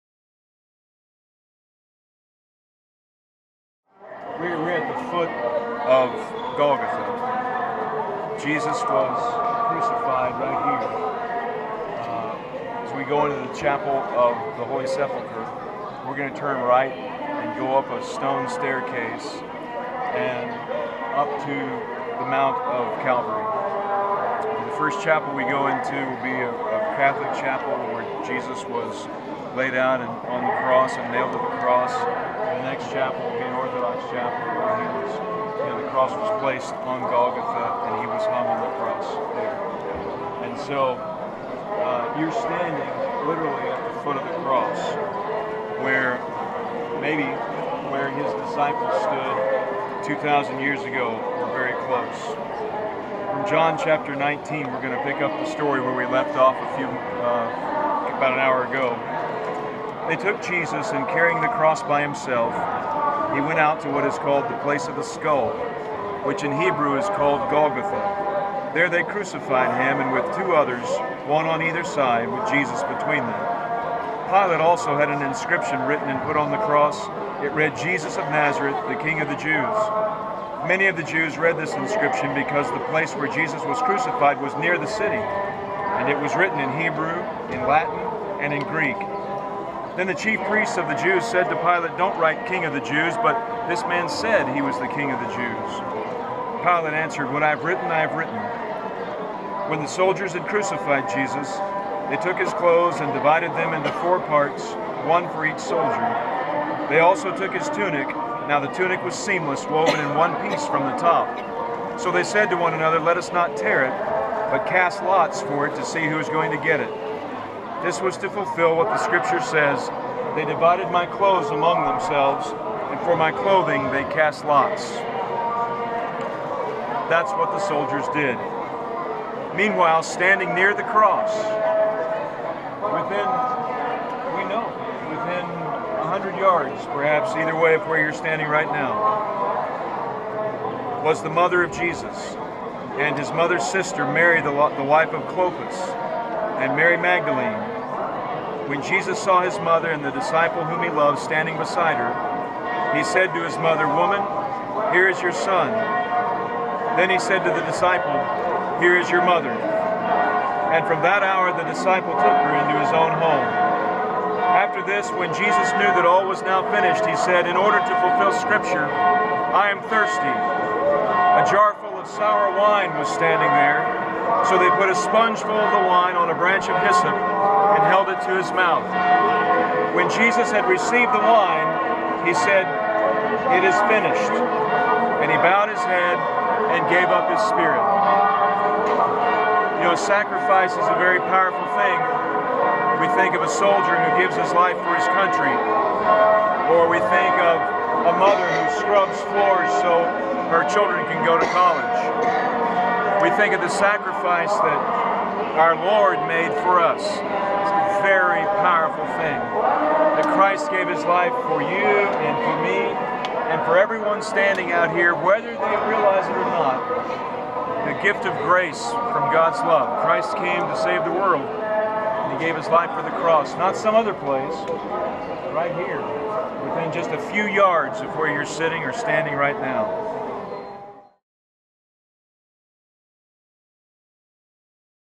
From Israel to Claremore: Part 15 - It is Finished
Pastor Ray shares with us from the plaza of the Church of the Holy Sepulcher in Jerusalem, Israel, just outside where Jesus was crucified.